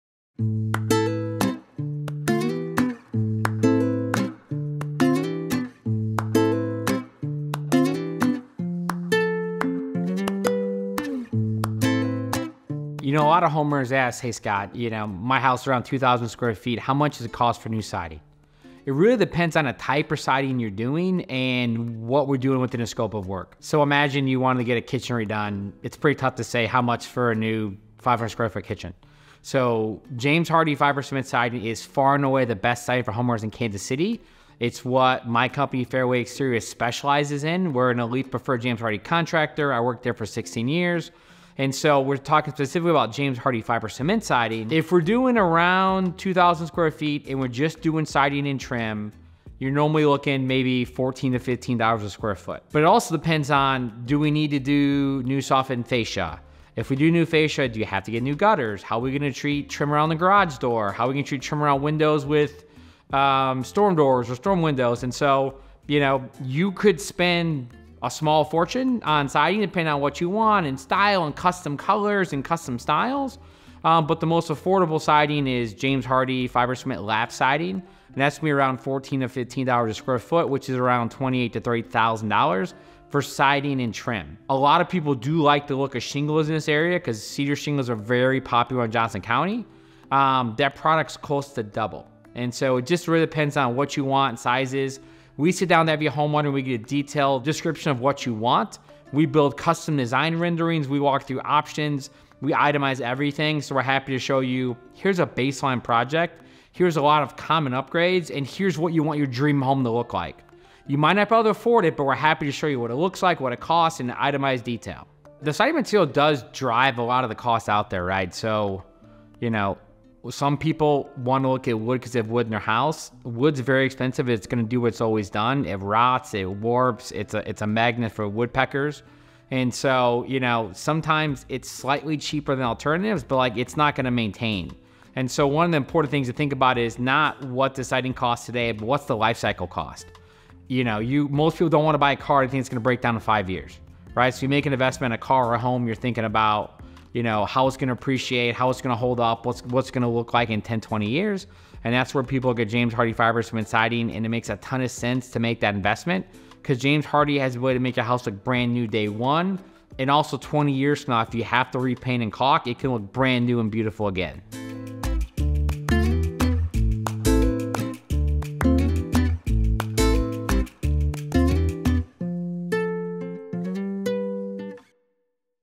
How Much Does Siding Cost for a 2,000 Sq Ft House? | Fairway Exteriors
Scott Neidow, the owner of Fairway Exteriors in Kansas City, breaks down the cost of siding for a 2,000 sq ft house. He discusses the factors that influence the pricing, including material choices, labor, and regional considerations. If you're planning a siding project, this video provides valuable insights to help you budget effectively.

0:00 - Introduction
0:29 - James Hardie Fiber Cement Siding
0:44 - Cost of Siding
1:05 - Choosing the Right Siding
1:35 - Custom Design and Pricing
1:55 - Considerations for Siding Material
2:15 - Long Term Investment